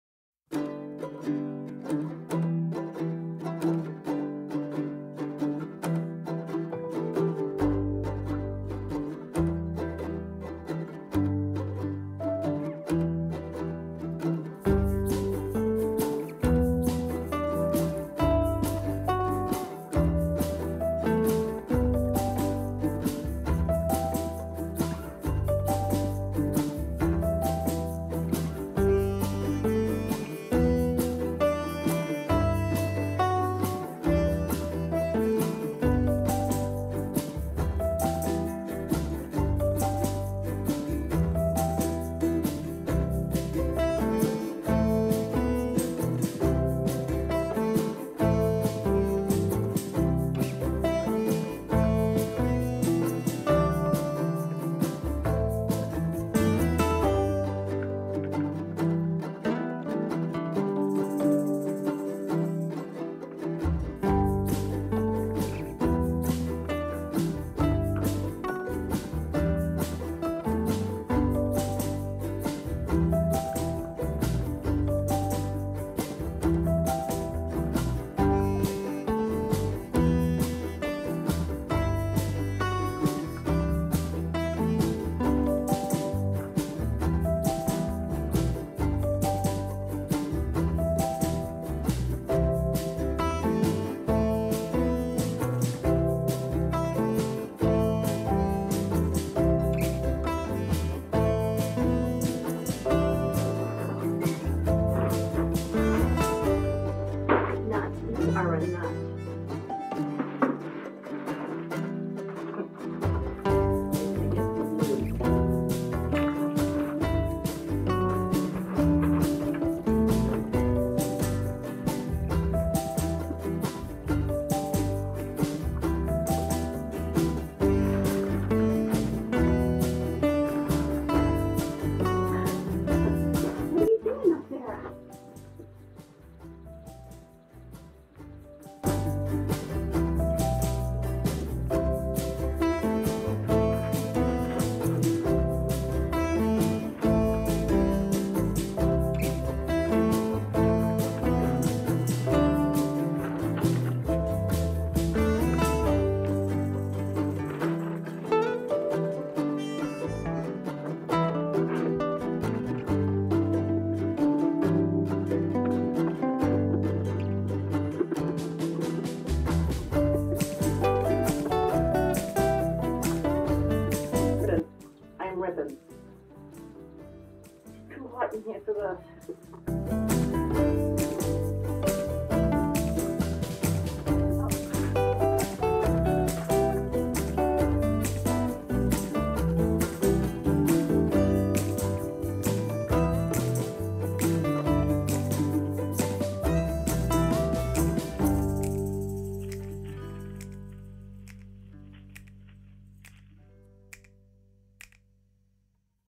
Slow night at the Raccoon Saloon but still a few cute moments
From new rubber duckies to Nutz showing out it was still a fun night.